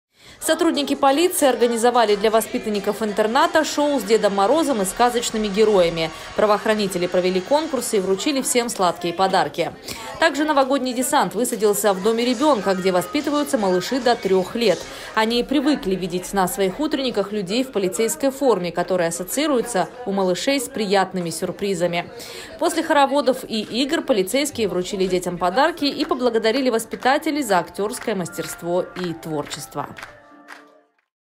Сочинские полицейские проводят акцию «Полицейский Дед Мороз»
Правоохранители посетили подшефную коррекционную школу- интернат №2 и дом ребенка.

Сотрудники полиции организовали для воспитанников интерната шоу с Дедом Морозом и сказочными героями. Правоохранители провели конкурсы и вручили всем сладкие подарки.

Также новогодний десант высадился в доме ребенка, где воспитываются малыши до трех лет. Они привыкли видеть на своих утренниках людей в полицейской форме, которые ассоциируются у малышей с приятными сюрпризами. После хороводов и игр полицейские вручили детям подарки и поблагодарили воспитателей за актерское мастерство и творчество.